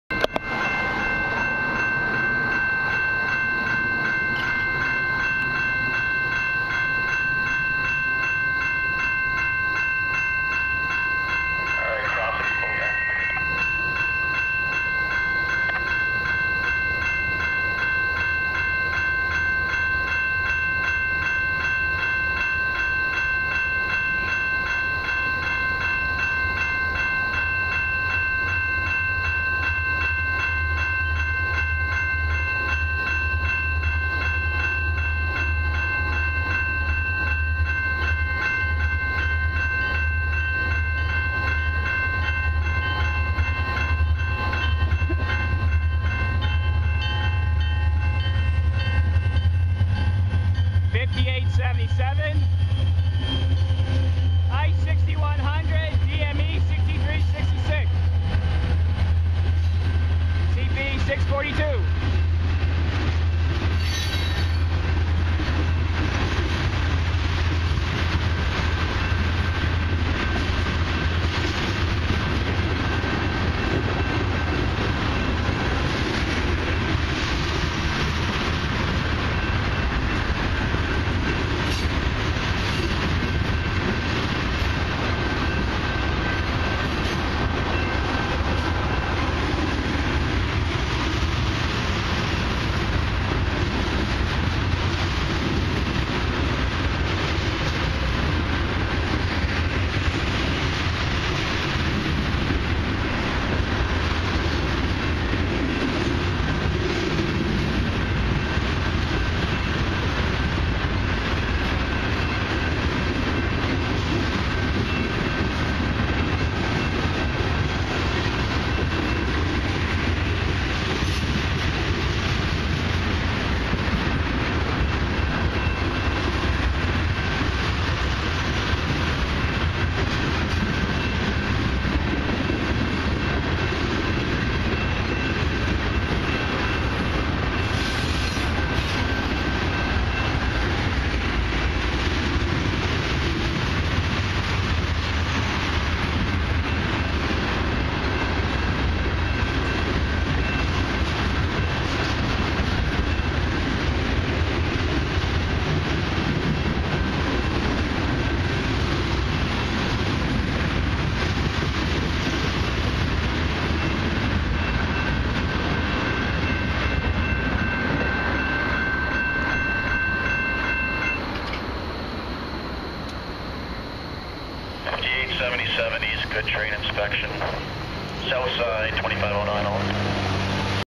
Night Shot/CP 642 w/ CP 5877, ICE 6100 & DME 6366 all hooked E.S. lead this long EB Loaded Ethanol
.train by Lisgar GO

here's my 3rd and final catch from yesterday.

and thanks to Tyler for the post on FB !

while i was waiting for this train, i was talking to a CP Foreman who knows me. you will see him to the left side of my video. hadnt seen him in a long time ! so it was good to see him.

anyways, after getting this train, nothing else was coming and so i packed up and booked it home.

video taken- Tuesday September 10th 2013 at 8:45pm
and as always, Comment & Enjoy !
thanks! =]